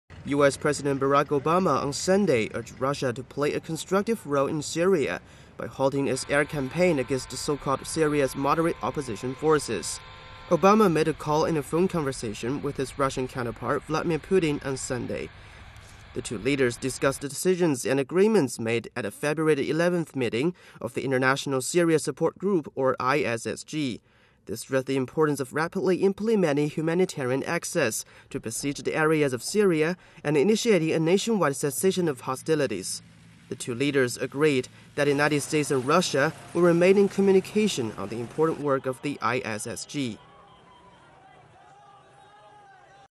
Obama urges Russia to halt airstrike against Syrian "moderate" rebels
U.S. President Barack Obama on Sunday urged Russia to play a constructive role in Syria by halting its air campaign against so-called Syria's moderate opposition forces.

Obama made the call in a phone conversation with his Russian counterpart Vladimir Putin on Sunday.